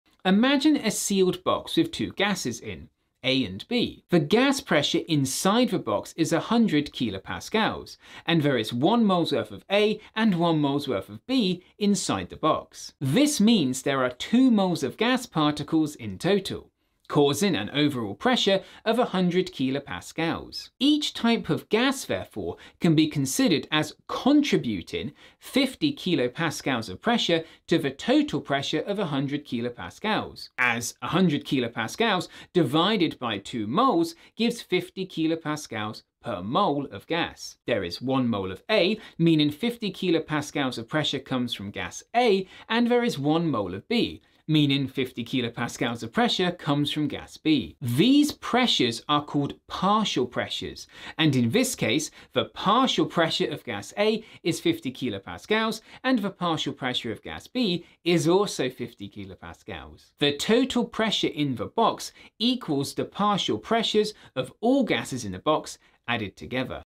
What is partial pressure of a gas?
A simple visual explanation of partial pressures: how different gases each contribute to the total pressure in a sealed container. Perfect for A-Level and IB Chemistry students learning gas laws and equilibrium.

for free AP, IB, A-level and NCERT (11 + 12) chemistry notes and resources!